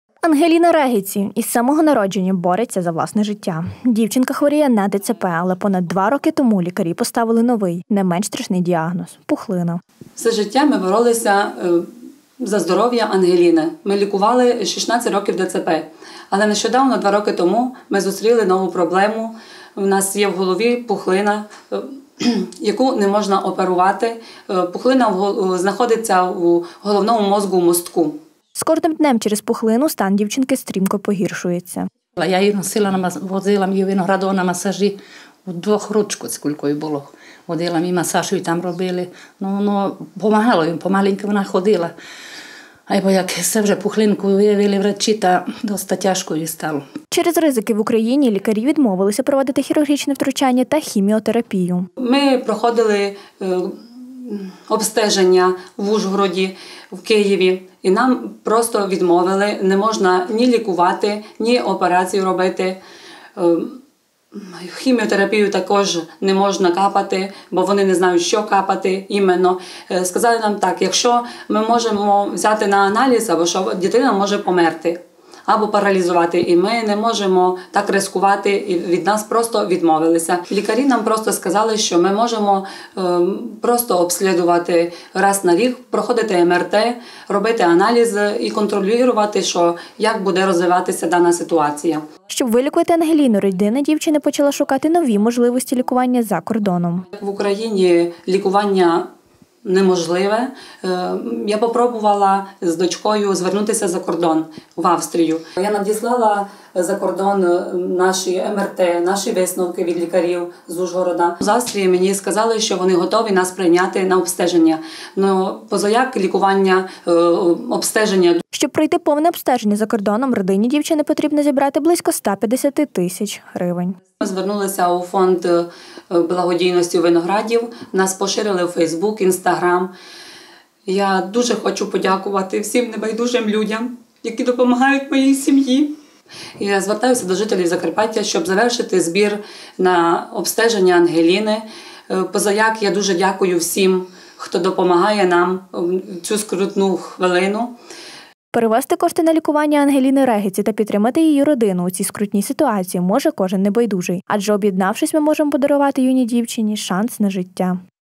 Пожертвуй кошти - врятуй життя
Збір коштів на обстеження Ангеліни Регеці триває. Дівчина з самого народження хворіє на ДЦП, але вже понад два роки, бореться з новою, не менш страшною хворобою – пухлиною стовбура головного мозку. За обстеження Ангеліни, для надання майбутнього лікування взялися лікарі з Австрії, тому родина дівчини потребує велику суму коштів в районі 150ти  тисяч. Ангеліна Регеці – із самого народження, бореться за власне життя. Дівчина хворіє на ДЦП, але понад 2 роки тому, лікарі постановили новий, не менш страшний діагноз – пухлина. З кожним днем, через пухлину, стан дівчинки стрімко погіршується. В Україні лікарі через ризики відмовилися проводити хірургічне  втручання та хіміотерапію. Щоб вилікувати Ангеліну, родина дівчини, почала шукати нові можливості лікування за кордоном. Щоб пройти повне обстеження за кордоном, родині дівчини потрібно зібрати близько 150-ти тисяч. Перевести кошти на лікування Ангеліни Регеці, та підтримати її родину у цій скрутній ситуації, може кожен небайдужий. Адже об’єднавшись ми можемо подарувати юній дівчині шанс на життя